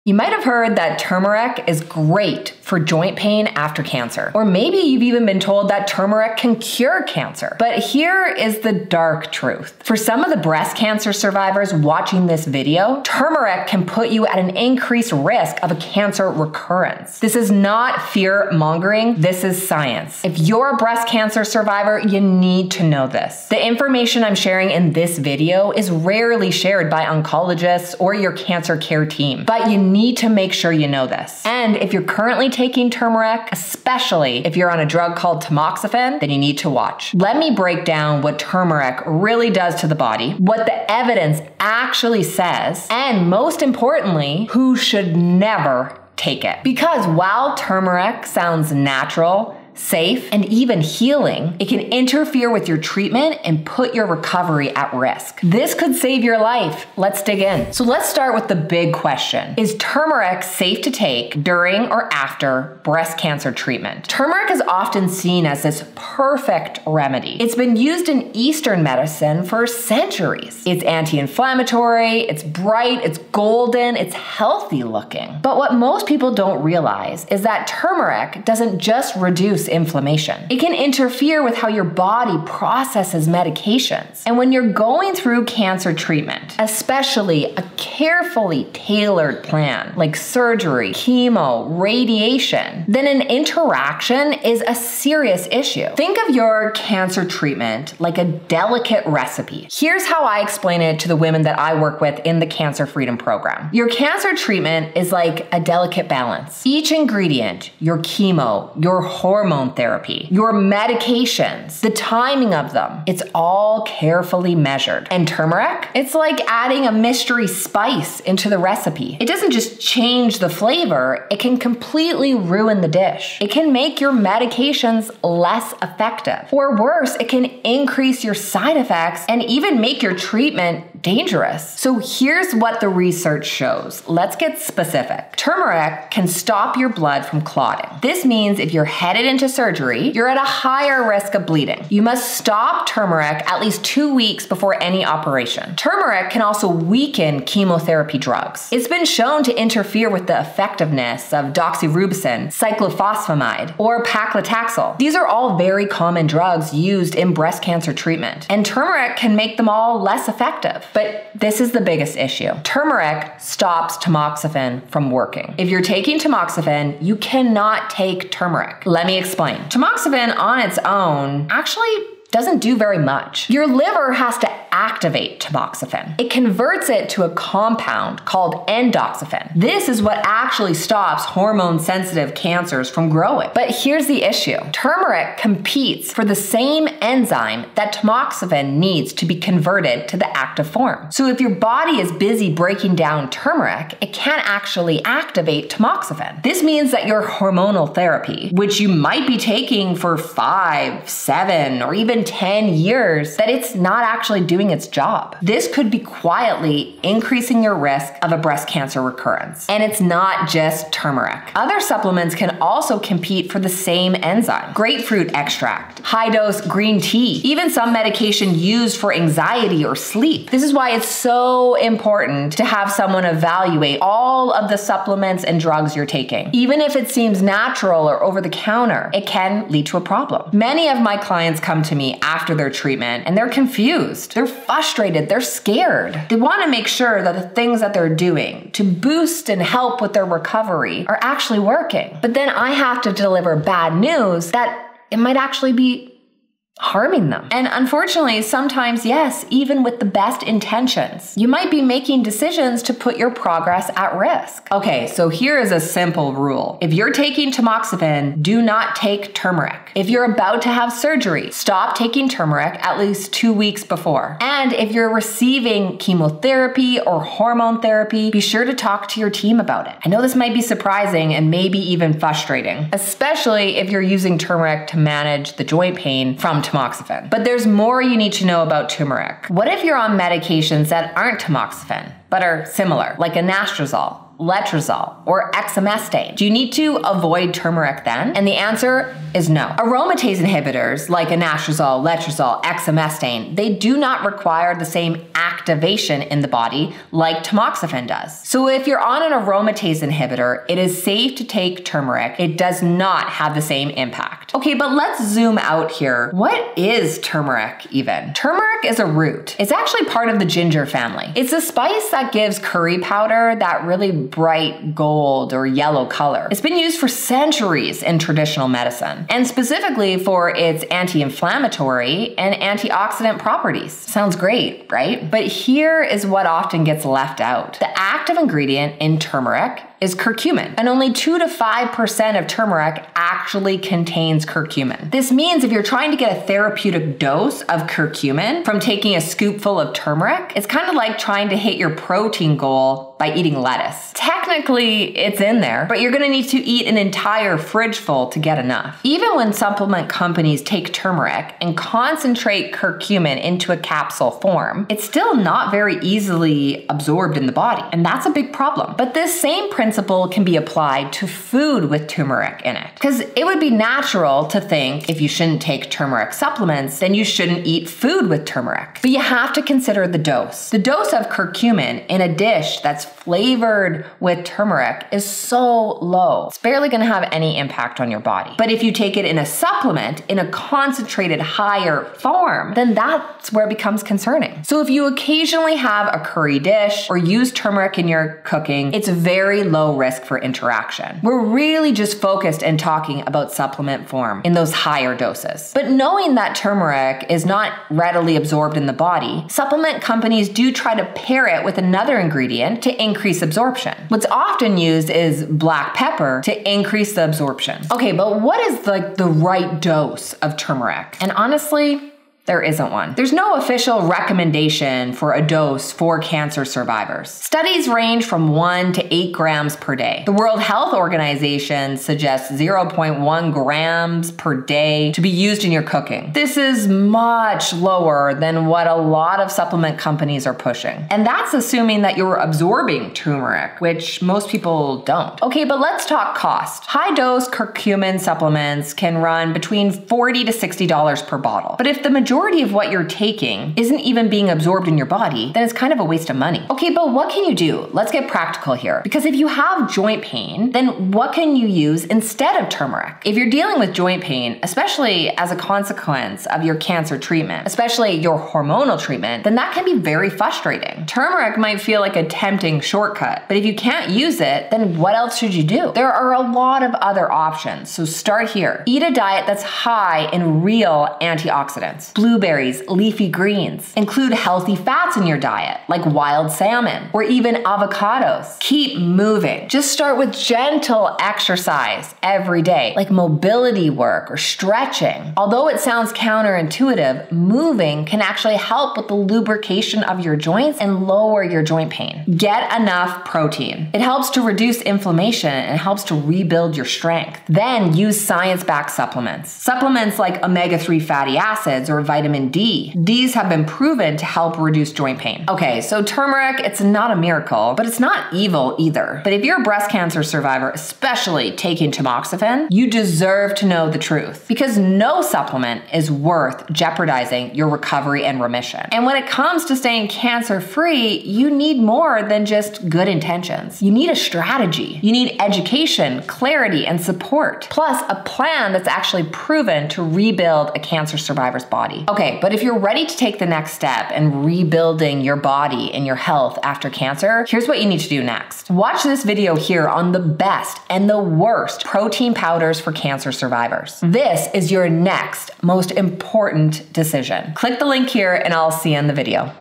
SHOCKING Truth – Breast Cancer & Turmeric (DANGER)
Turmeric is marketed as a natural, anti-inflammatory, cancer-fighting superfood.

🛑 But it has a DARK SIDE…

If you're a breast cancer survivor on Tamoxifen — turmeric could increase your risk of recurrence.

In this video, I reveal the science behind turmeric, curcumin, and why this supplement could interfere with your cancer treatment.

This is what you deserve to know before you take another pill.

Let me show you how it’s done -